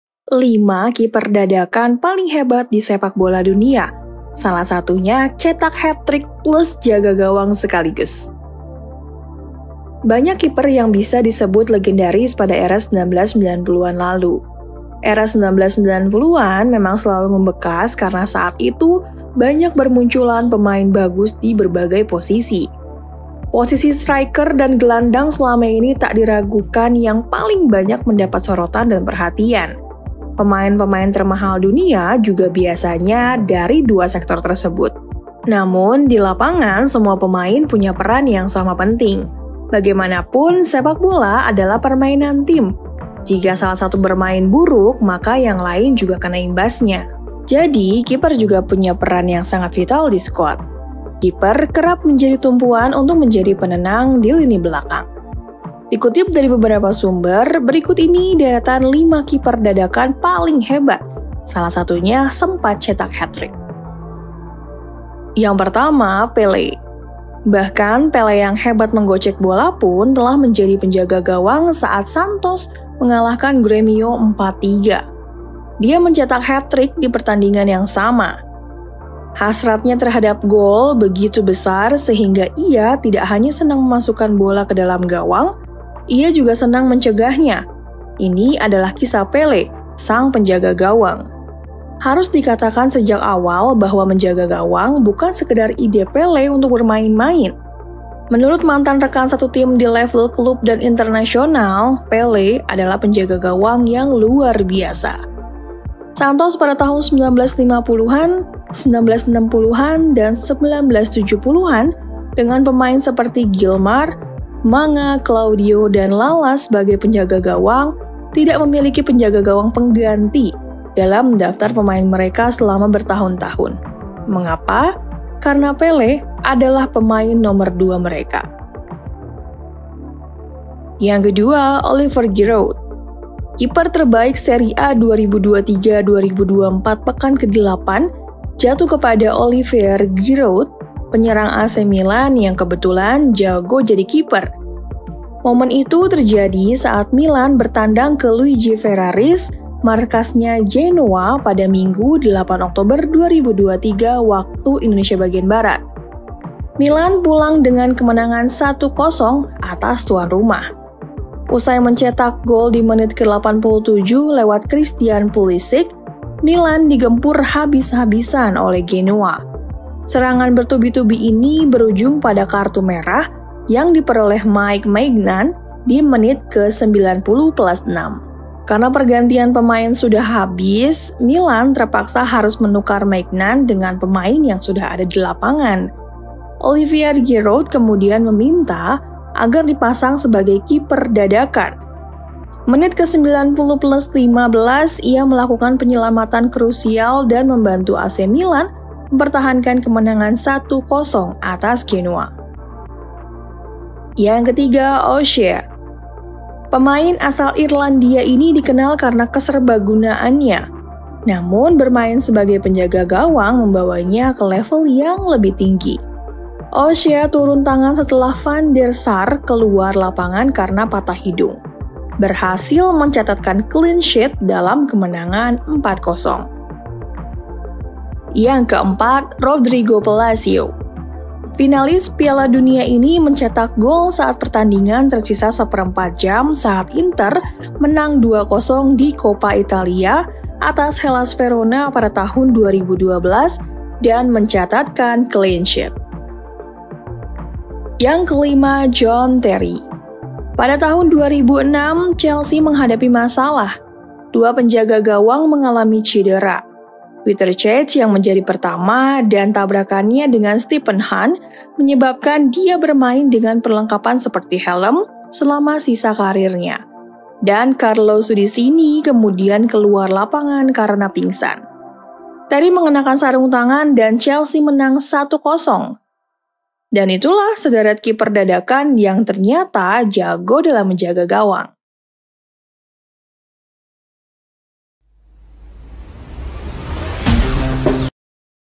5 Kiper Dadakan Paling Hebat di Sepak Bola Dunia, Salah Satunya Cetak Hattrick Plus Jaga Gawang
Banyak kiper yang bisa disebut legendaris pada era 1990-an lalu. Era 1990-an memang selalu membekas karena saat itu banyak bermunculan pemain bagus di berbagai posisi.

Posisi striker dan gelandang selama ini tak diragukan yang paling banyak mendapat sorotan dan perhatian. Pemain-pemain termahal dunia juga biasanya dari dua sektor tersebut.

Namun, di lapangan semua pemain punya peran yang sama penting. Bagaiamana pun, sepak bola adalah permainan tim. Jika salah satu bermain buruk, maka yang lain juga kena imbasnya.

Jadi, kiper juga punya peran yang sangat vital di skuat. Kiper kerap menjadi tumpuan untuk menjadi penenang di lini belakang.

Dikutip dari beberapa sumber, Berikut ini Deretan 5 Kiper Dadakan Paling Hebat, Salah Satunya Sempat Cetak Hattrick.